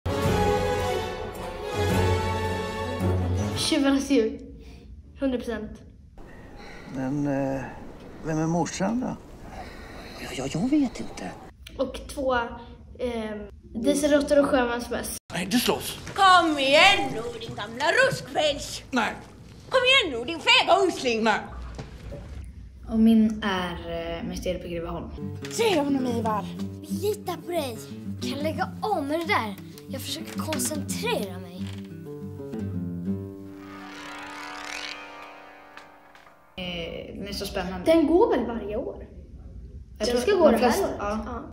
Kerstin & Xavier från julkalendern svarar på vilken som är deras bästa julkalender!
Håller du med?